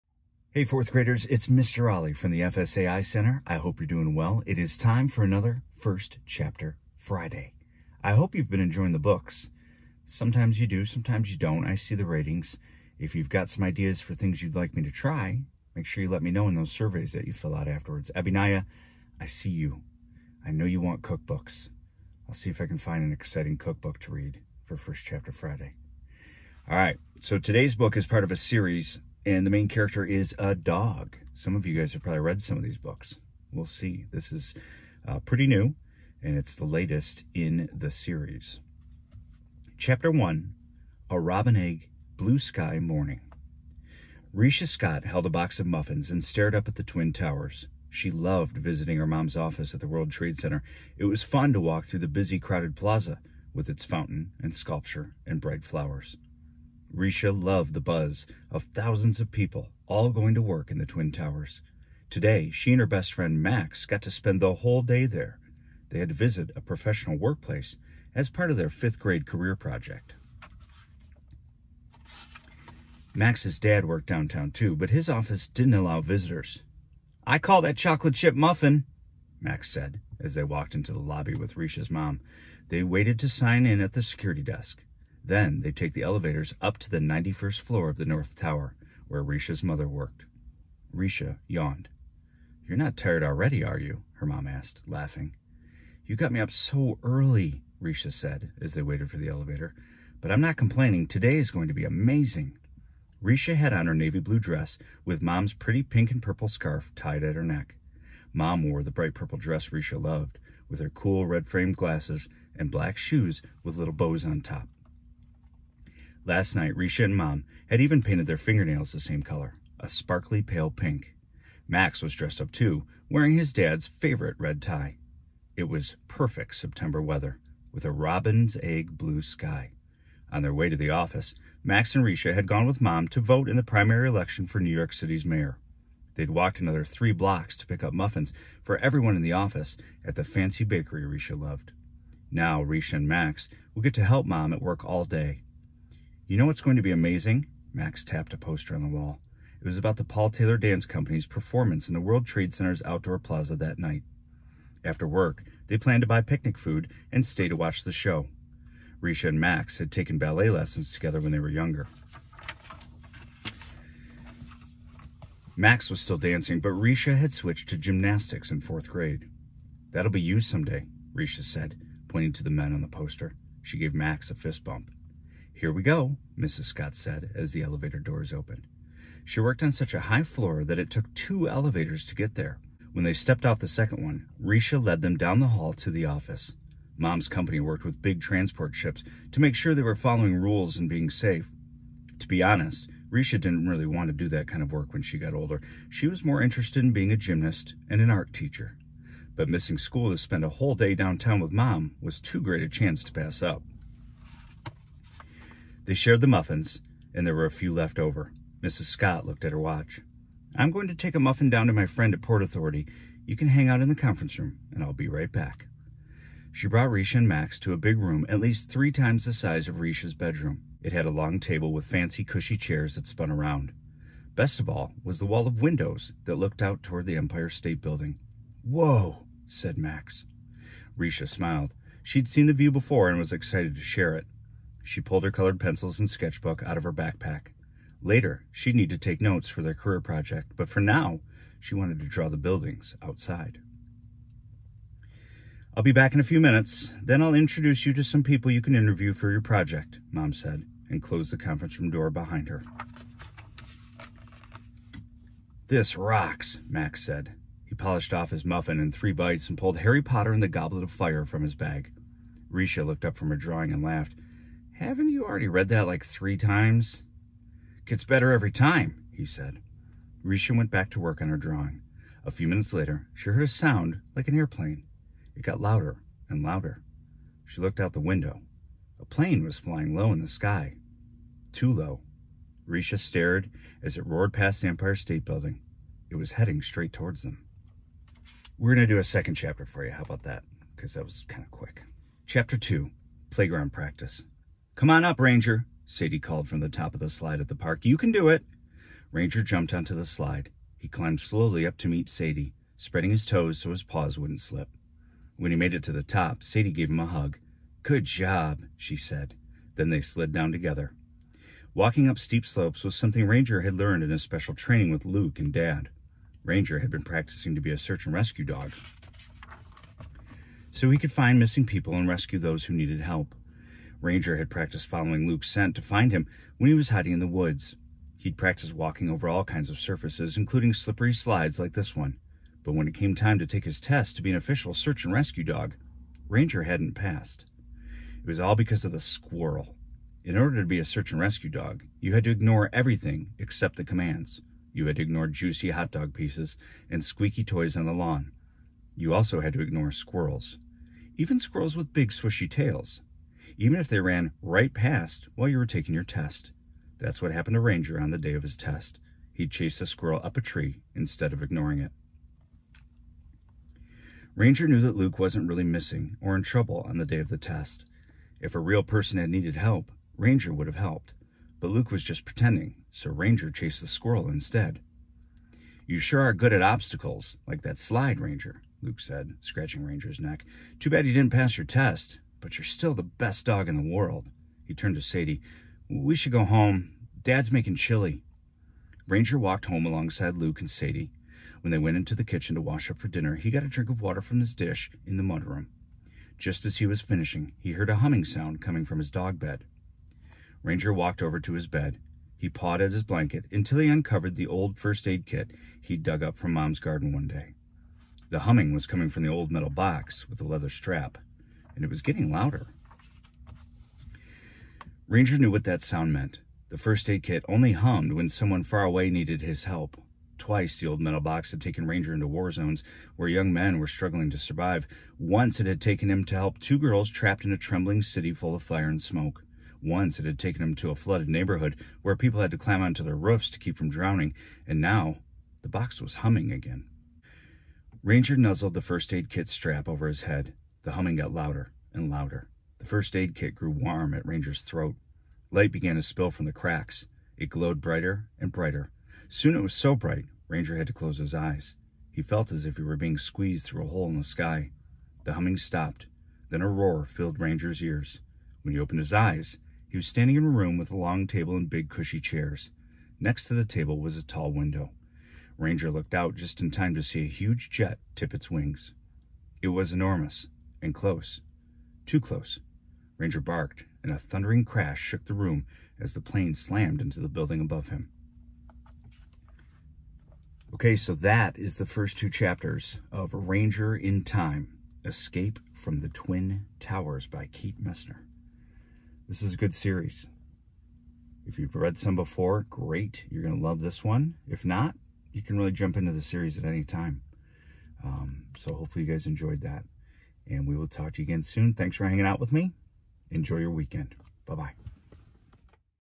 First Chapter Friday - Ranger In Time Escape from the Twin Towers by Kate Messner
Our fantastic 4th Grade teachers have asked me to be a part of "First Chapter Fridays." Each week I will be sharing the first chapter of one of my favorite books in an effort to get kids excited about new material.

I'll post them here after they've watched the video on Friday.